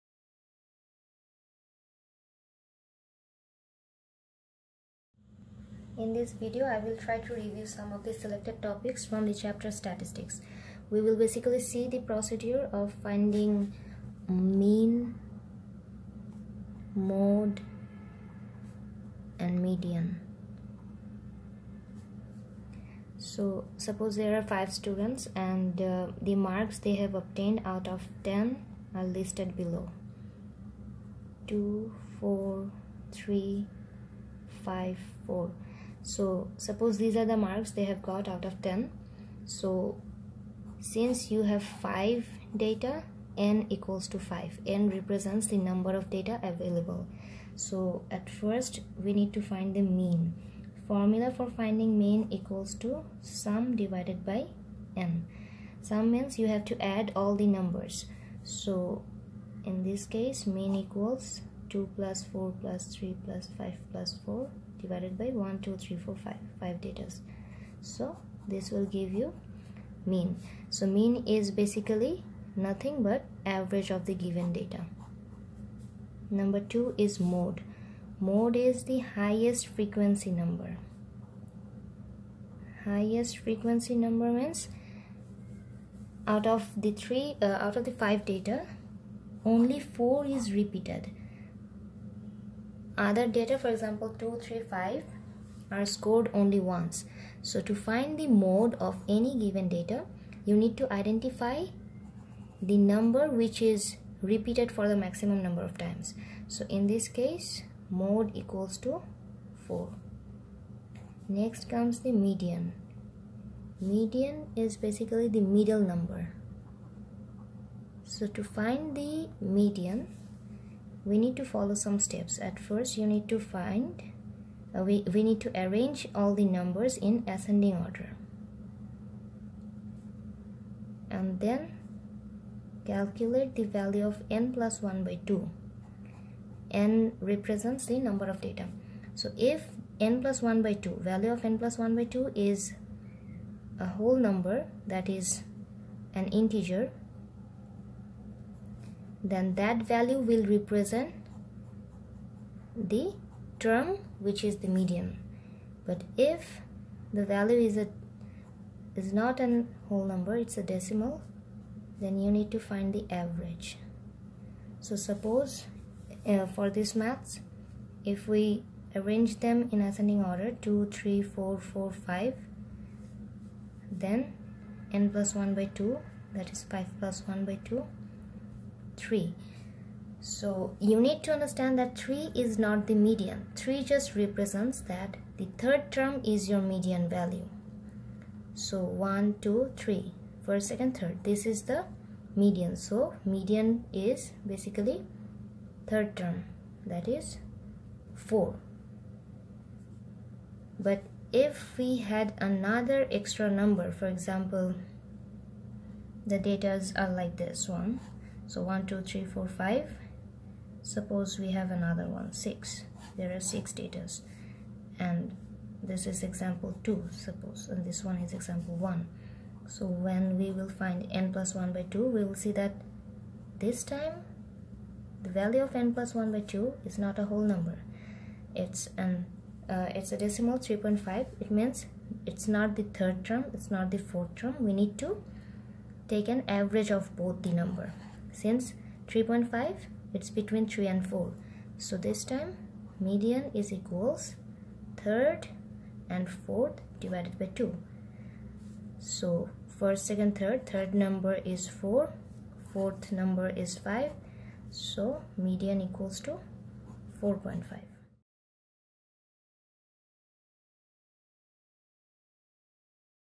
Statistics 1.0
Calculation of mean, mode & median from raw data
EDEXCEL IGCSE MATHEMATICS B syllabus has this topic.